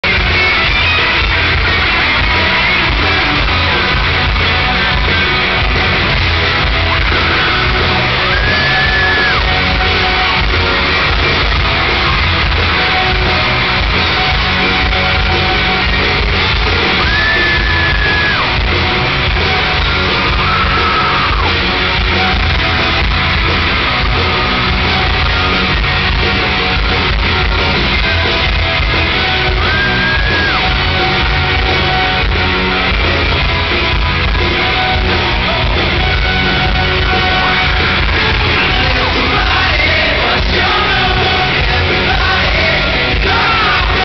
Jonas Brothers Concert - Foam!
The Jonas Brothers shoot foam all over the audience at the BOK Center June 22, 2009.